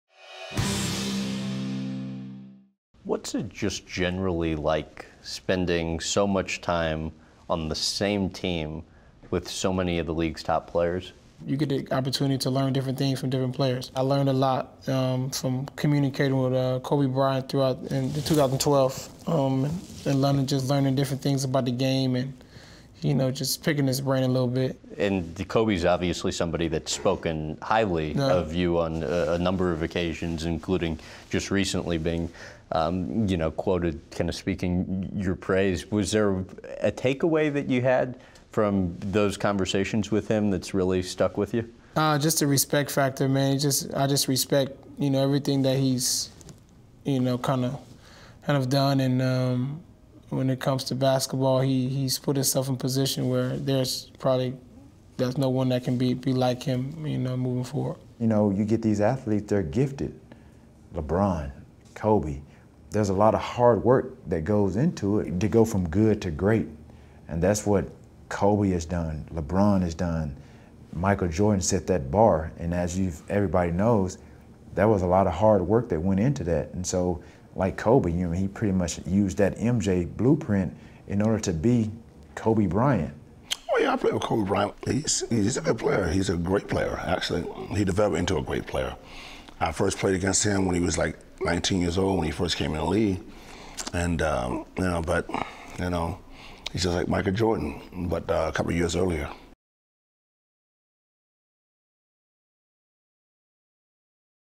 Westbrook, Owens, Rodman on Kobe Bryant
Star athletes Russell Westbrook, Terrell Owens, and Dennis Rodman speak about Kobe Bryant's talent, work ethic,  and the impact he had one the game of basketball.

Episode debuted nationwide in 2020. Interviews were filmed prior to Kobe Bryant's death.